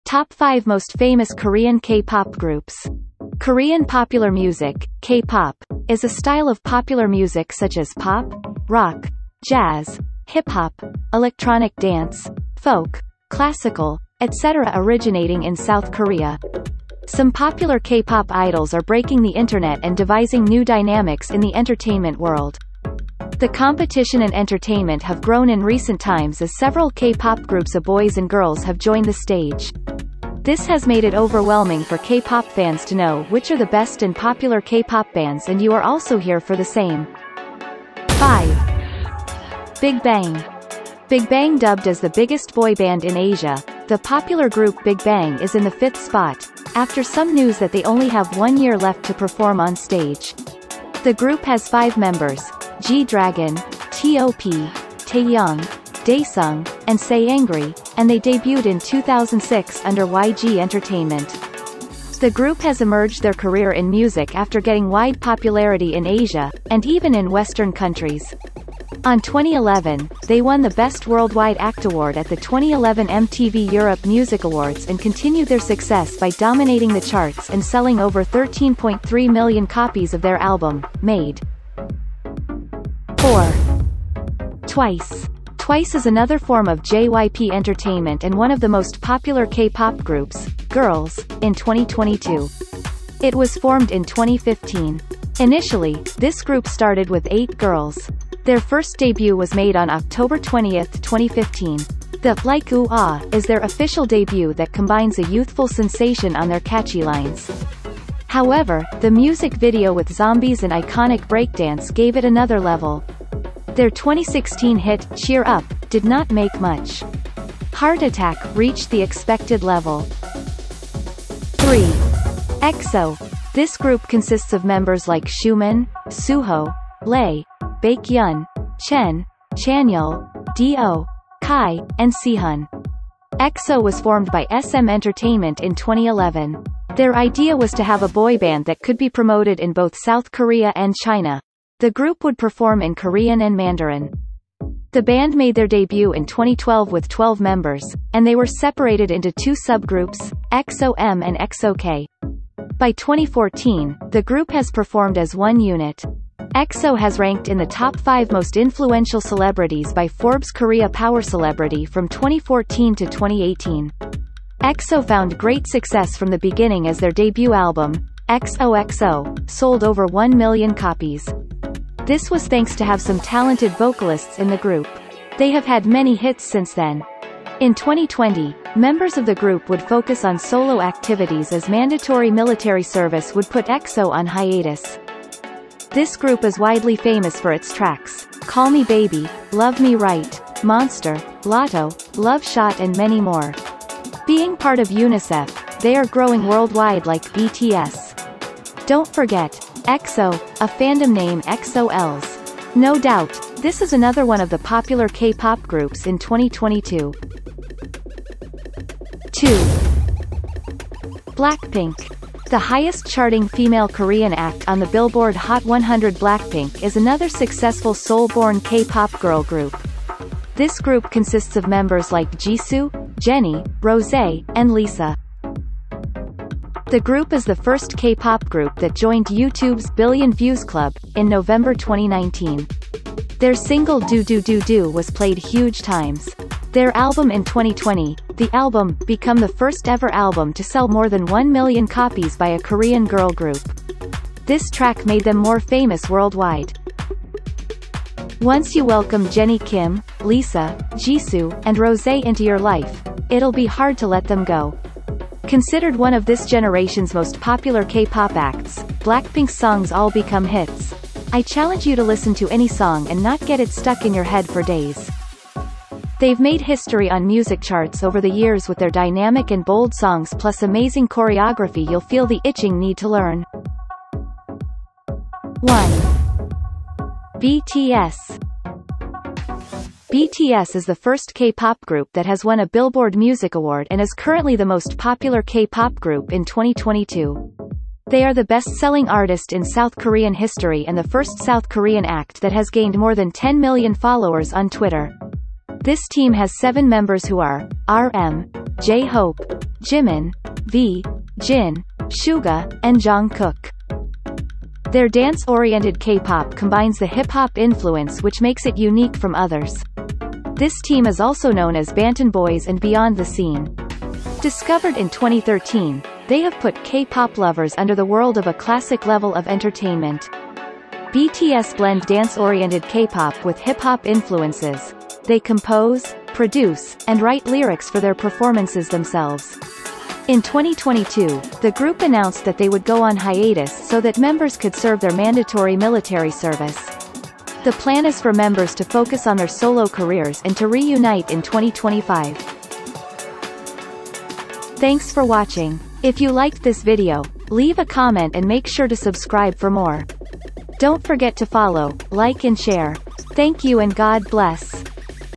MAGUGULAT KA KUNG SINO ANG UNA! SOBRANG GALING! | Bigbang | Twice | EXO | BLACKPINK | BTS
TOP 5 MOST POPULAR K-POP GROUPS!

Korean popular music (K-pop) is a style of popular music such as pop, rock, jazz, hip hop, electronic dance, folk, classical, etc. originating in South Korea. Some popular K-pop idols are breaking the internet and devising new dynamics in the entertainment world. The competition and entertainment have grown in recent times as several K-pop groups of boys and girls have joined the stage. This has made it overwhelming for K-pop fans to know which are the best and popular K-pop bands and you are also here for the same.